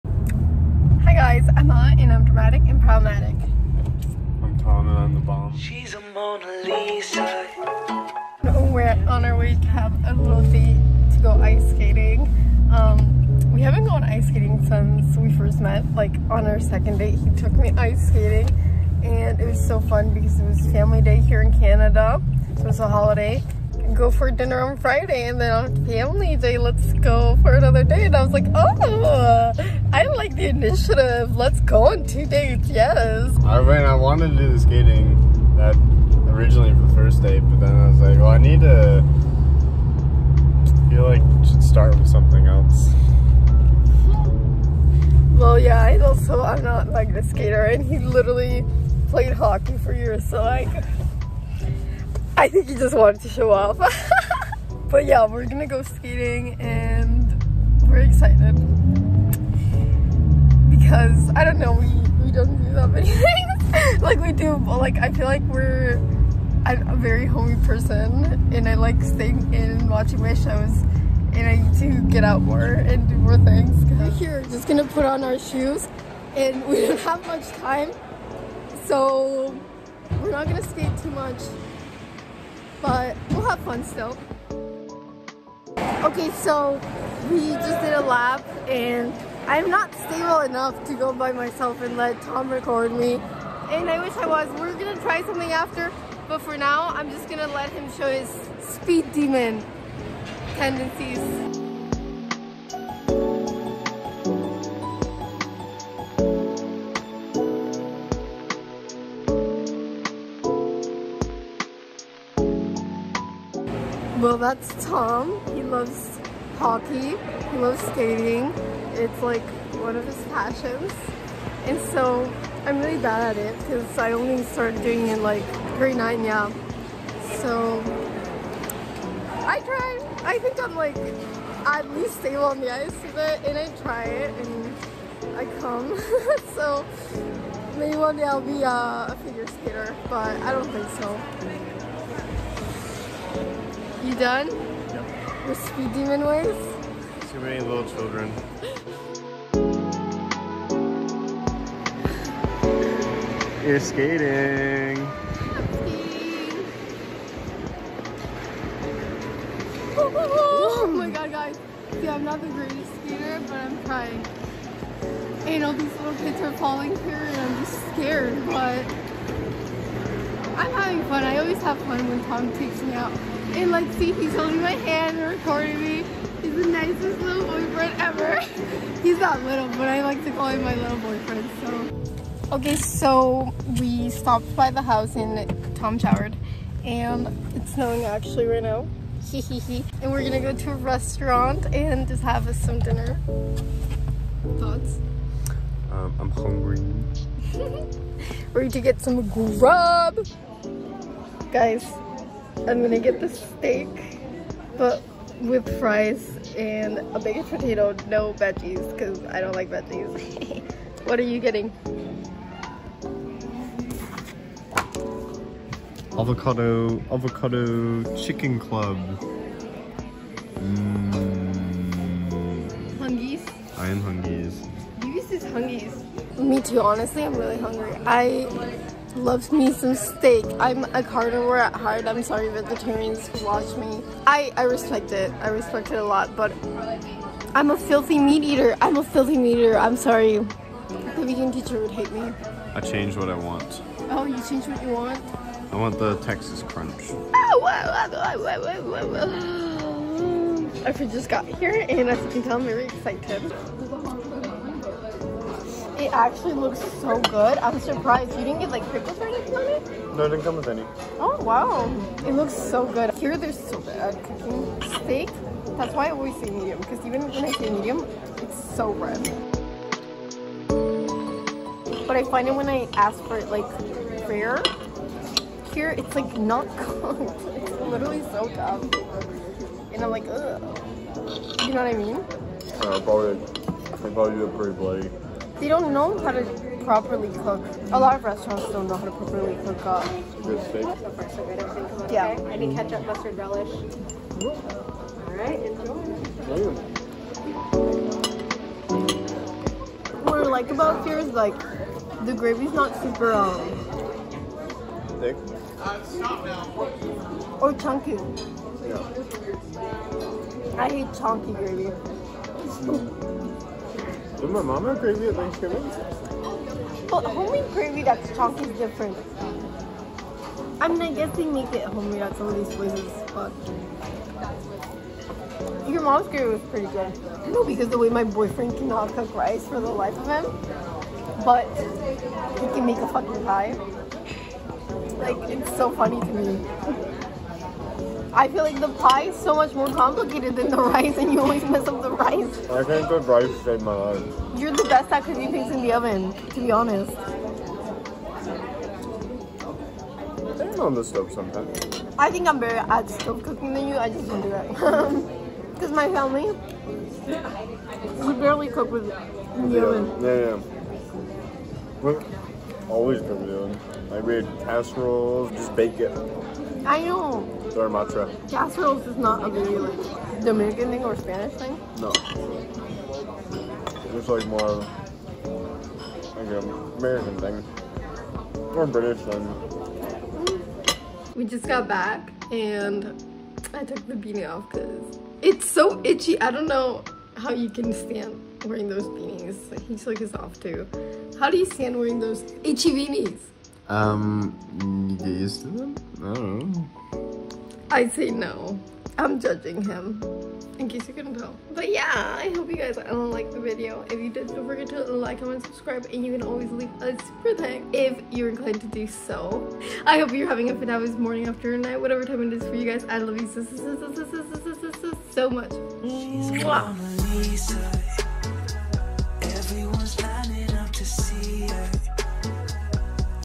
winter date night vlog (with my fiancé ❤️)
we had a really fun date night on this date! skating and steak goes well together i suppose haha. anyways really love this mans and even though this wasn't on valentines day i wanted to celebrate it with y'all by showing you our winter themed little date.